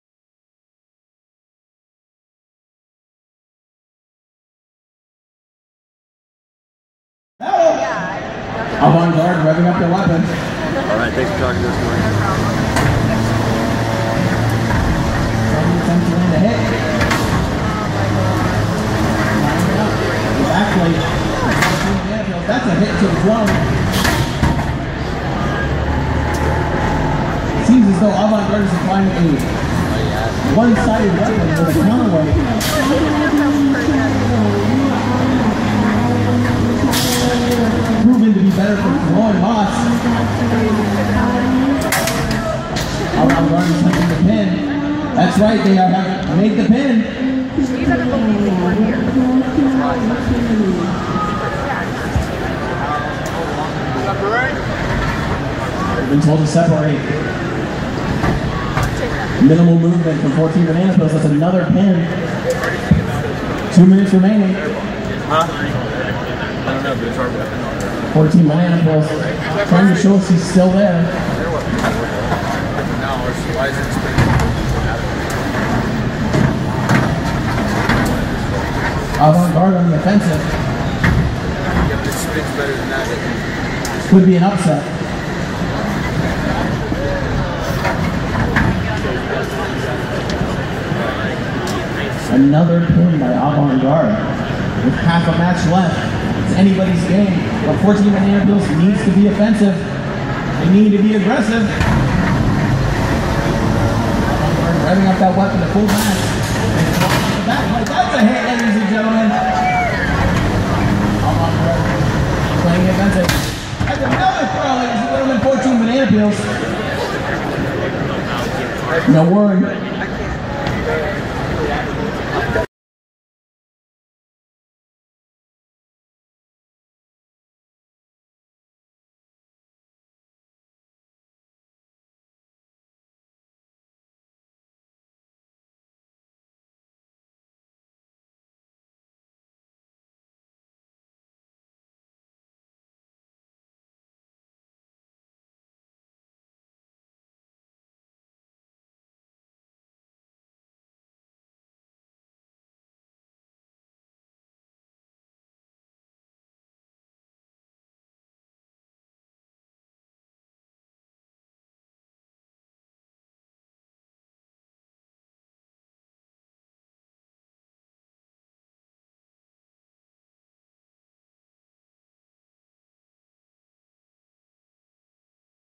2017 Bots IQ - Cal HS - 3rd Match
California HS: 14 Banana Peels vs. Greater Latrobe: Avant Garde